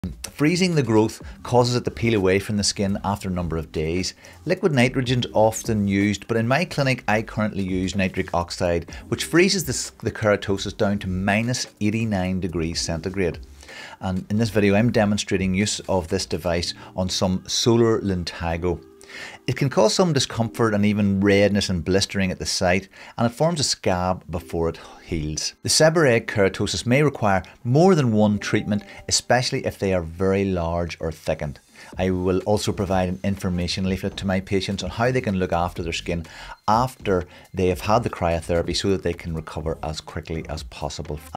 Cryotherapy demonstration Solar Lentigo: dermatologist explains
Here a quick demo of cryotherapy in action.